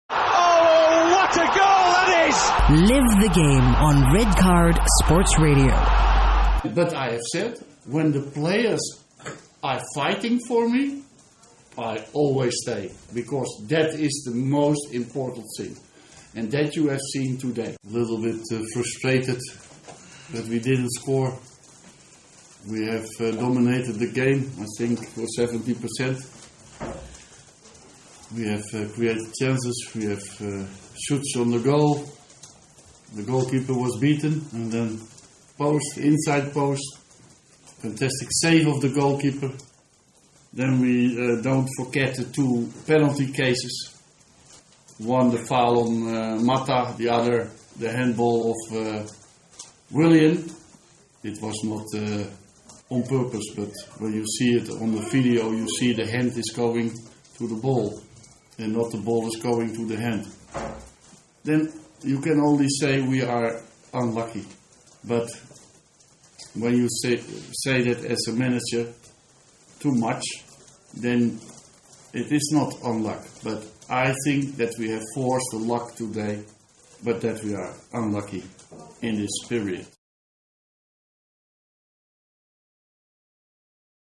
Louis Van Gaal: I Will Stay If The Players Fight For Me
Listen to Louis Van Gaal's comments after the goalless draw against Chelsea at Stamford Bridge.